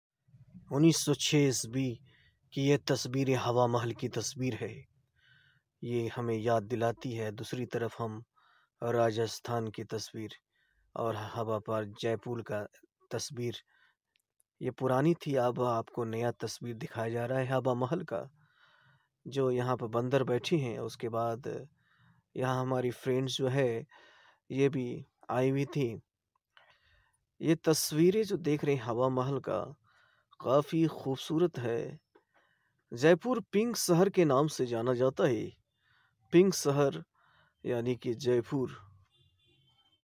1906 Ki yadgaar tashwir Hawa Mahal ka Aur Dusri tashvir new photo Hawa Mahal ka tashvir
1906 Ki yadgaar tashwir Hawa Mahal ka Aur Dusri tashvir new photo Hawa Mahal ka tashvir. Created by VideoShow